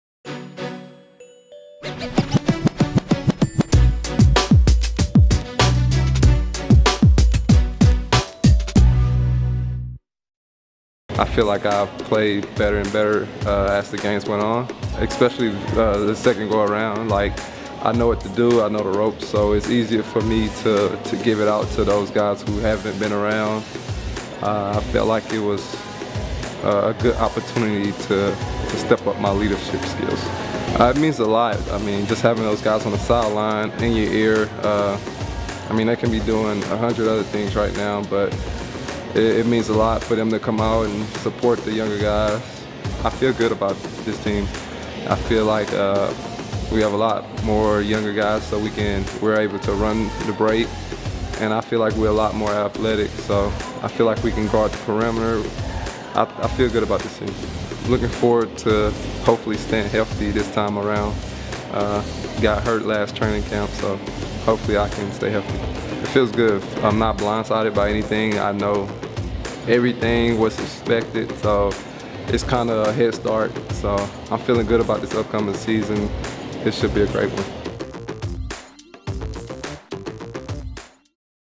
Markel Brown Talks Summer League Experience
The Nets guard talks about his experience playing NBA Summer League for the second time around and having more of a leadership role with the squad.

We Are Brooklyn